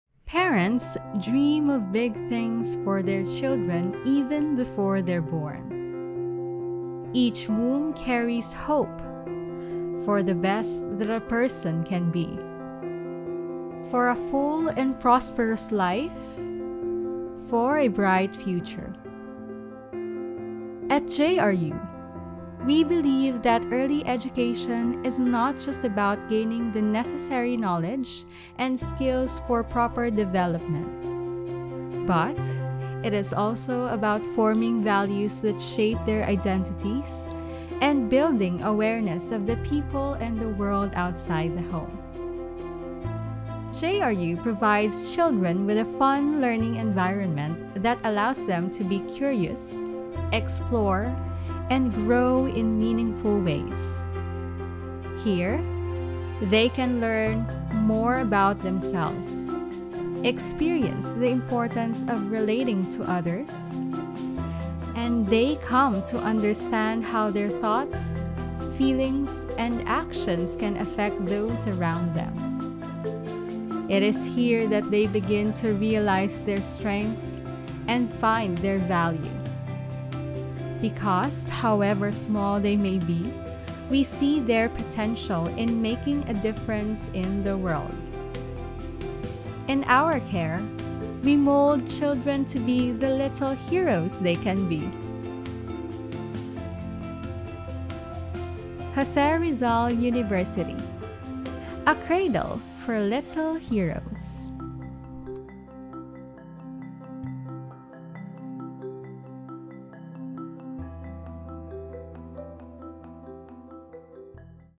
Heroes, no matter how small | Kindergarten and Elementary School
Parents dream of big things for their children even before they're born. Each womb carries hope — for the best that a person can be, for a bright future.

At JRU, we consider that early education is not only about acquiring the skills and knowledge required for healthy development but also about helping children acquire the values that will define their identities and enhance their understanding of others and the world at large.

Because however small they may be, we recognize their capacity to change the world. We shape children in our care to become the little heroes they can be.